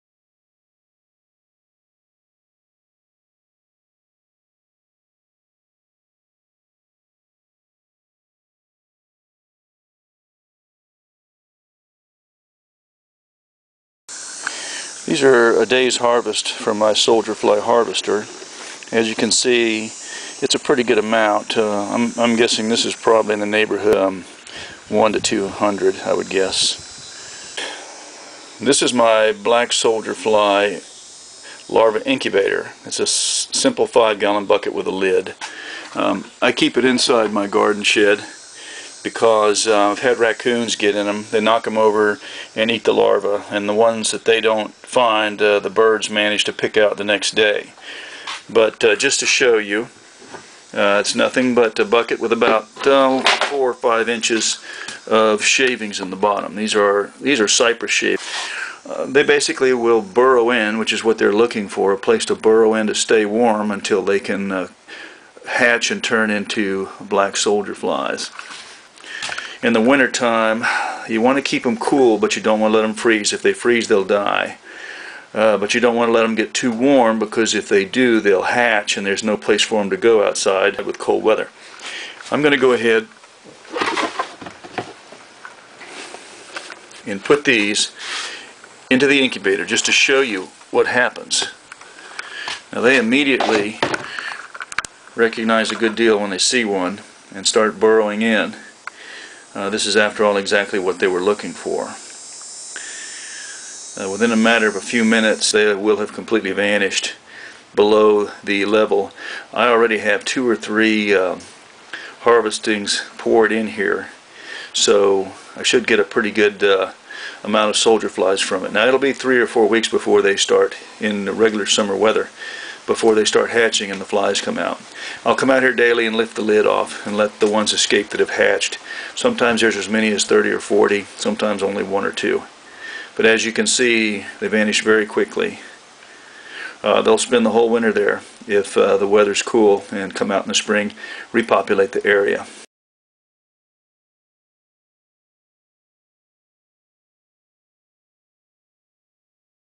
The BSF Incubator - BoonJon system
A simple BSF incubator for use in developing a black soldier fly population in the area.